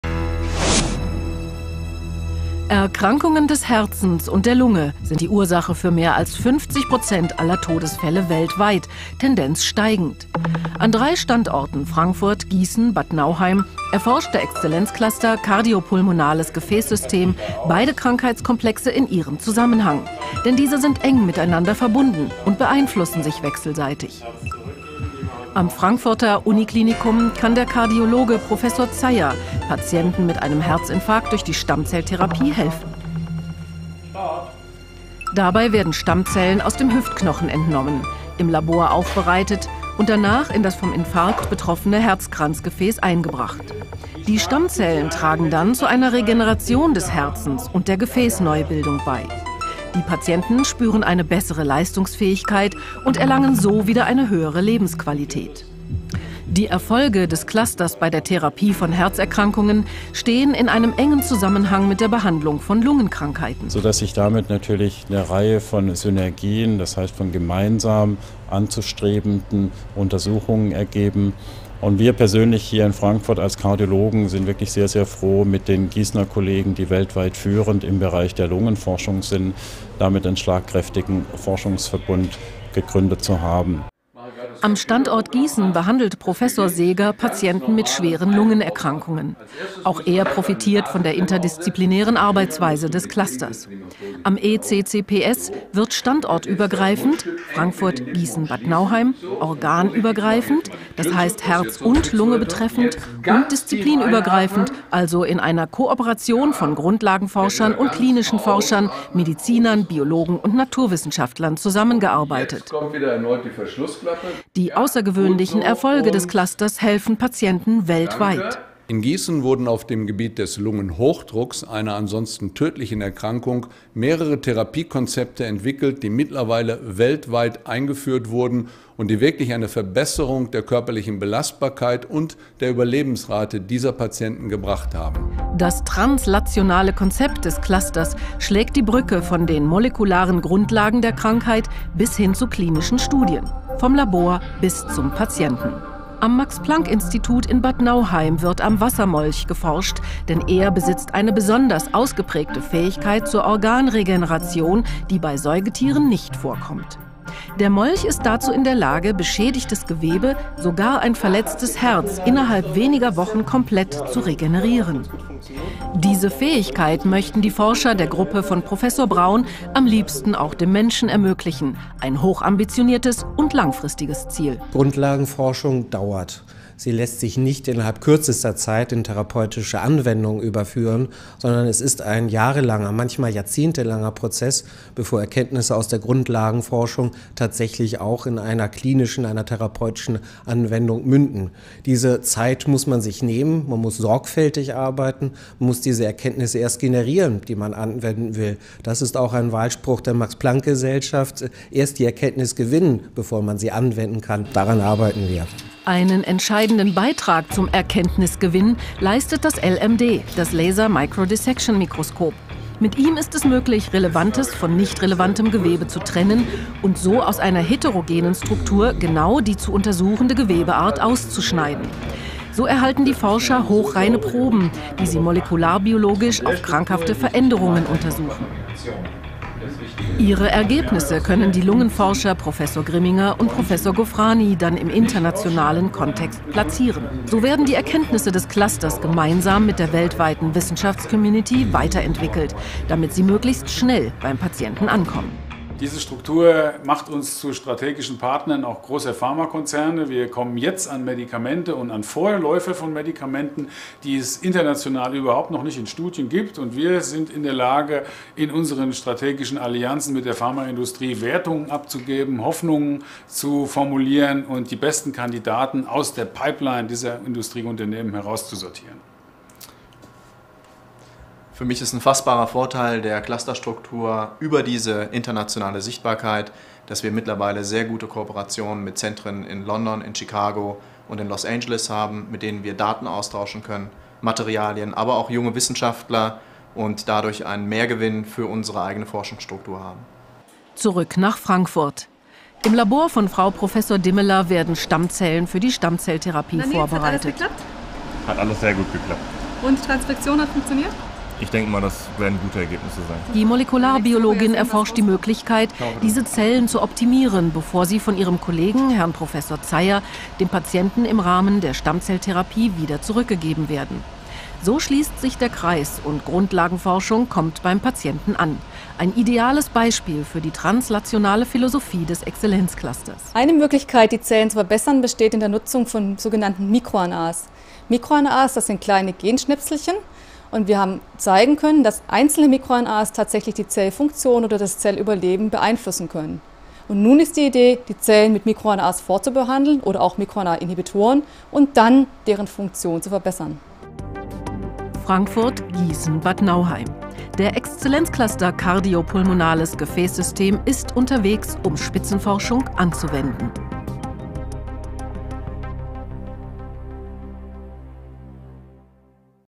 Exzellenzinitiative: Exzellenzcluster "Kardiopulmonales Gefäßsystem"
Themengebiet des Excellenzclusters Kardiopulmonales Gefäßsystem sind die Erforschung der Erkrankungen von Herz und Lunge.

Die Forschung reicht von der Untersuchung der Stammzellen in Entwicklungs- und Reparaturprozessen und molekularen Regulationsmechanismen über Tiermodelle bis hin zur klinischen Anwendung. Zum Beispiel beherrscht der Molch die Fähigkeit zur Organregeneration, die bei Säugetieren nicht vorkommt. Ein langfristiges Ziel des Clusters ist es diese Regeneration auch dem Menschen zu ermöglichen. Bis dahin sorgen Entwicklungen im Bereich der Stammzelltherapie, sowie auf dem Gebiet der molekularen Regulation für die Entstehung neuer Therapiekonzepte für betroffene Patienten.

Die translationale, standortübergreifende Zusammenarbeit von Frankfurt, Giessen und Bad Nauheim schafft Synergien, die den Fortschritt in der Therapie von Herz-Lungenerkrankungen ermöglichen, beziehungsweise beschleunigen werden. Die disziplinübergreifende Kooperation von Grundlagenforschern, vorklinischen und klinischen Forschern vollendet das Konzept, welches neue Therapien direkt vom Labor zum Patienten bringt.